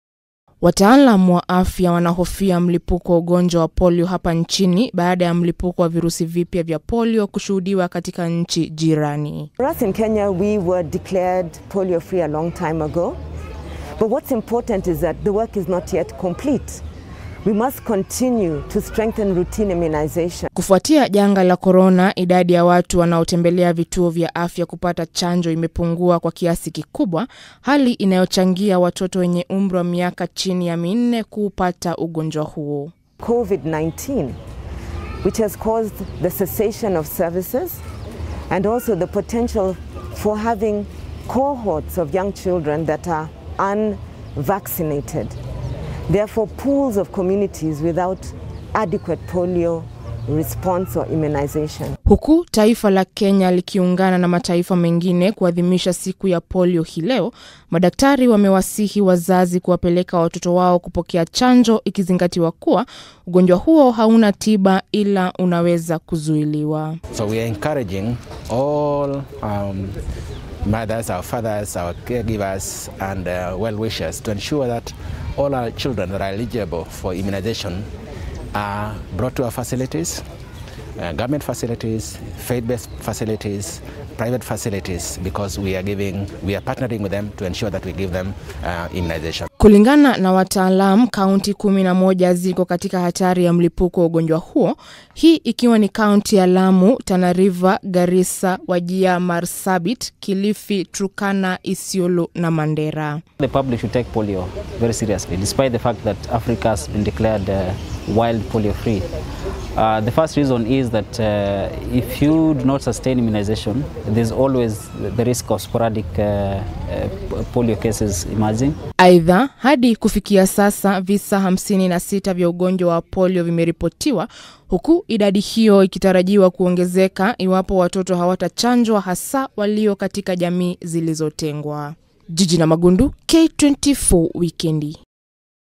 Wataalamu wa afya watoa onyo huenda kukashuhudiwa mlipuko wa Polio
Wataalamu wameonya kuwa huenda ugonjwa wa polio ukalipuka tena nchini kutokana na idadi ndogo ya watoto wanaopokea chanjo dhidi ya ugonjwa huo